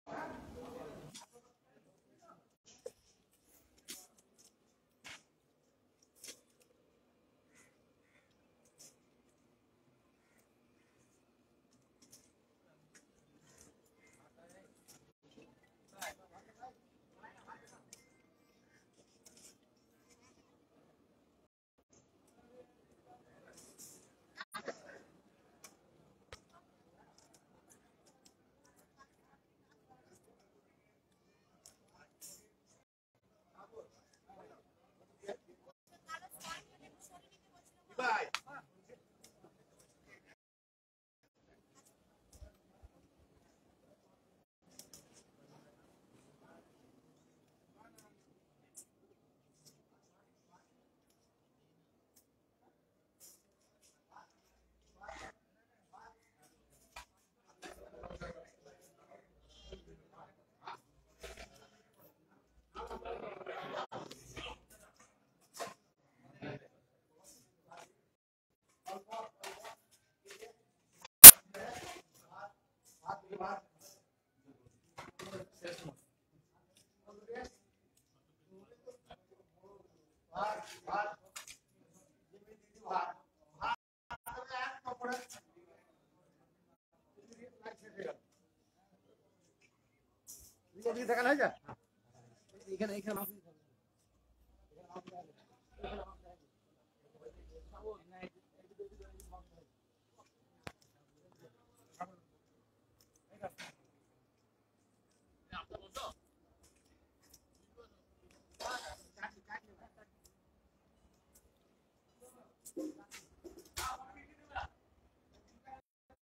MLA Nasiruddin Ahmed takes lunch at a local residents house in Barachandghar gram panchayat
Amid Panchayat Polls, Bengal's ruling party Trinamool Congress has launched a campaign named as 'Didir Suraksha Kawach'. By the campaign, 'Didir Doot' will visit door to door in rural areas of Bengal and connect with grassroot people.